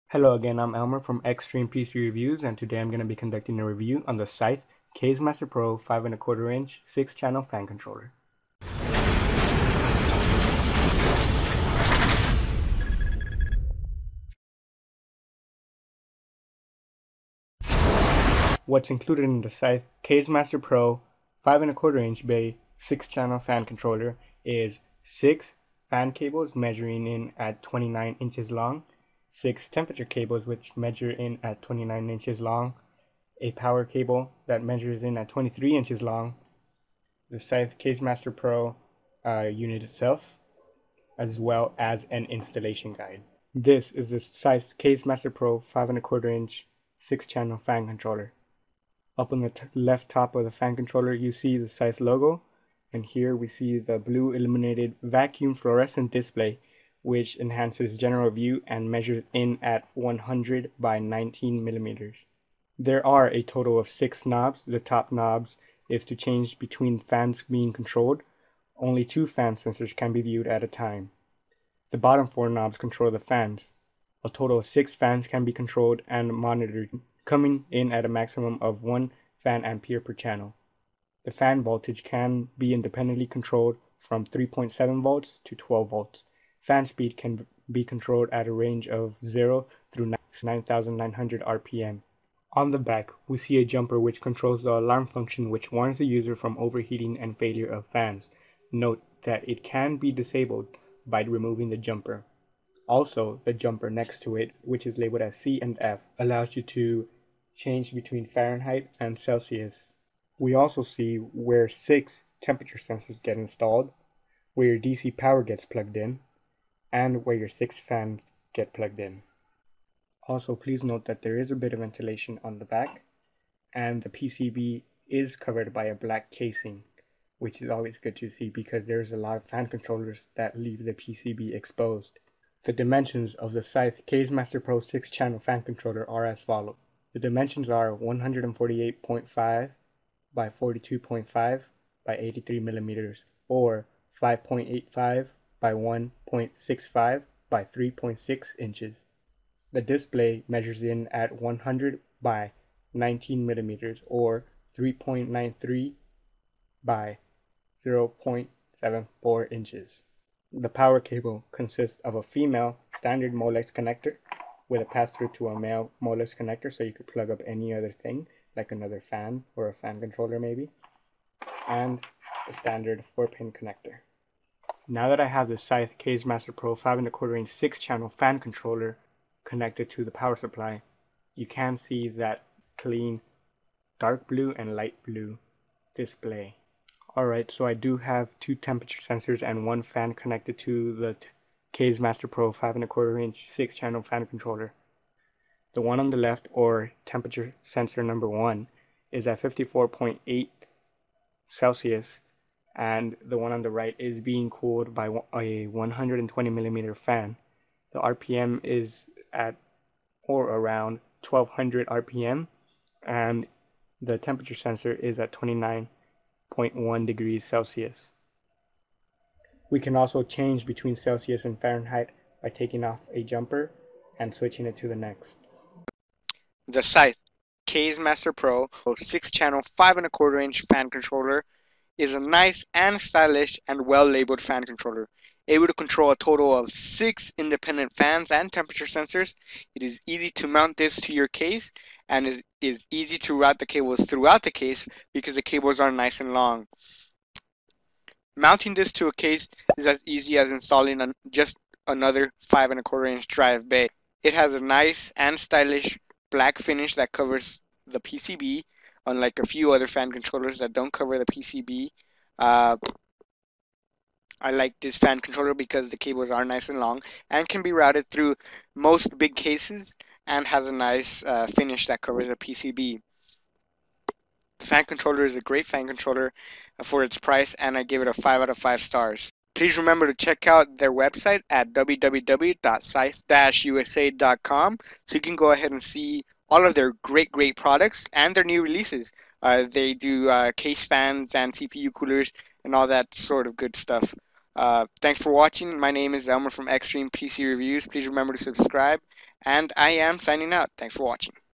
Review of the Scythe Kaze Master Pro 5.25" 6 Channel Fan Controller
Click for more info please.

This product is from Scythe, you can visit their

This fan controller allows the user to control a total of 6 fans and temperature sensors. Allowing the user to change the RPM to make sure you are getting a very quiet PC or a High Performance PC. The interface is nice and clean and Scythe went ahead and covered the PCB which i really like. Installation is quite simple , it has 4 mounting holes (2 on each side) for a easy mounting to the case. The cables provided with this Fan Controller, are nice and long,which makes it easier to route the cables through the case. This product is great for its price range and can be found on the website above.
Please remember to check out uranoob2007 and Cosmoknight74